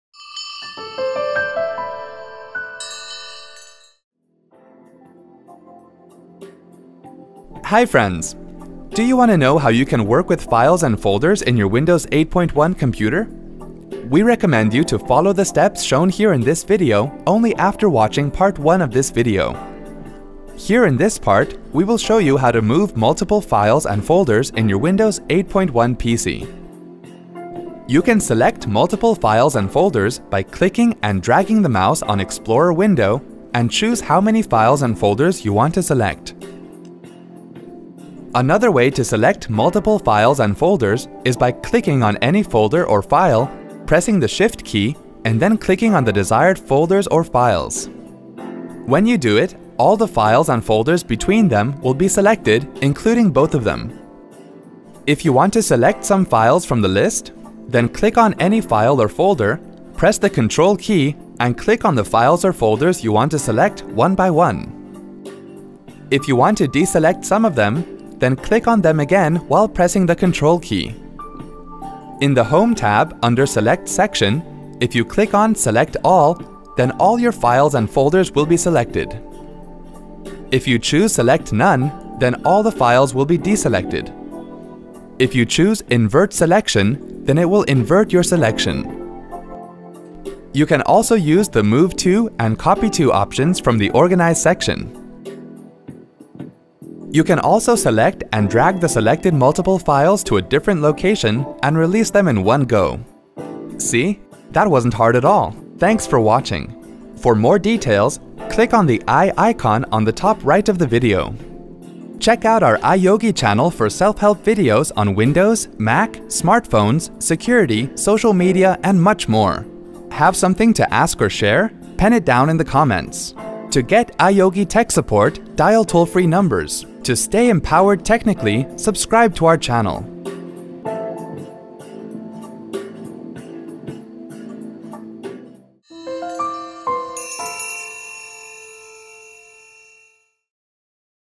How to select multiple Files and Folders in Windows® 8.1 (Part 2) :Tutorial :Tutorial
If you want to know how you can select multiple Files and Folders in Windows® 8.1, watch this video and follow the steps to do so.